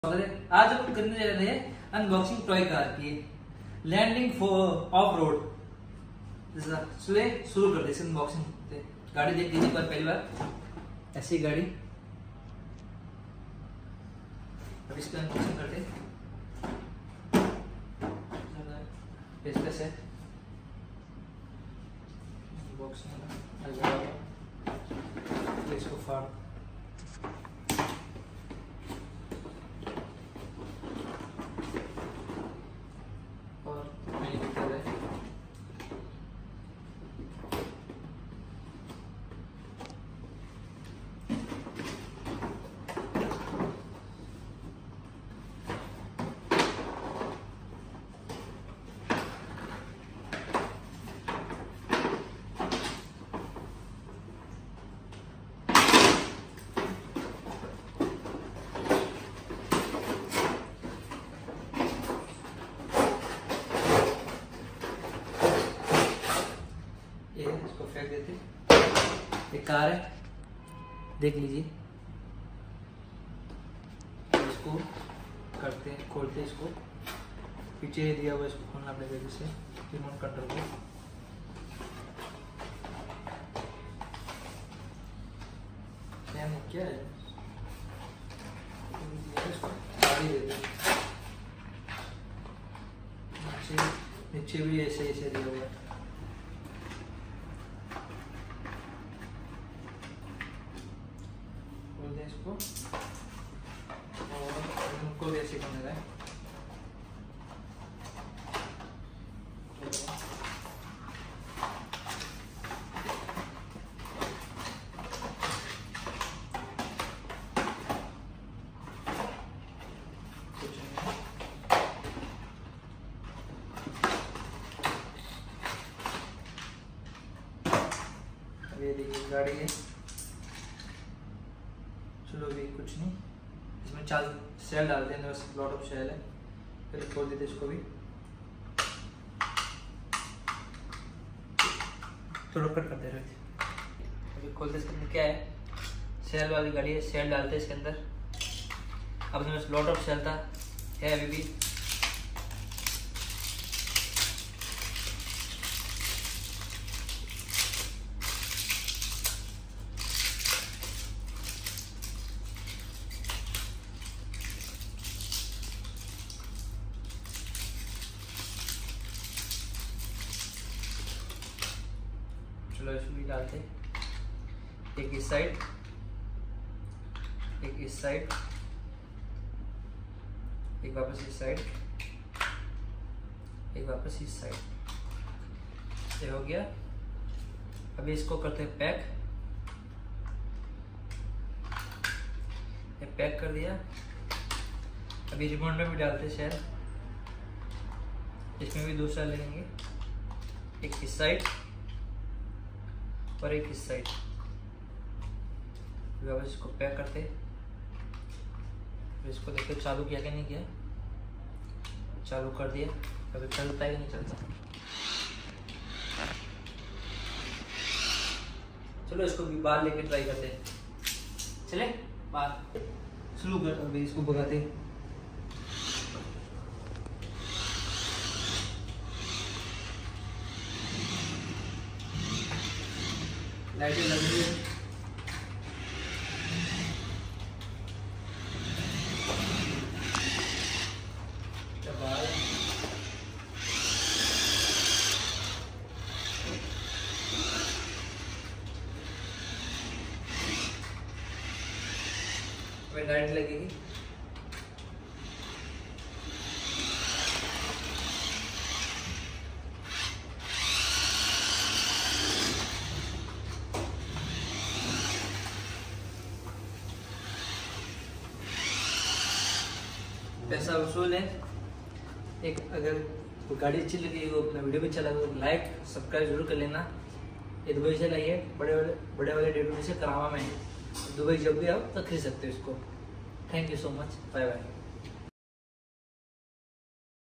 LANDING OFF ROAD TOY CAR ,cheap price in Dubai
Lending off road toy car full function: forward reverse,Let Turn ,Right ,Turn ,Lights.
All terrain remote controlled vehicle
Extreme Adventure
Sports car
Day to Day karama Dubai
Every item is 1 to 20 ad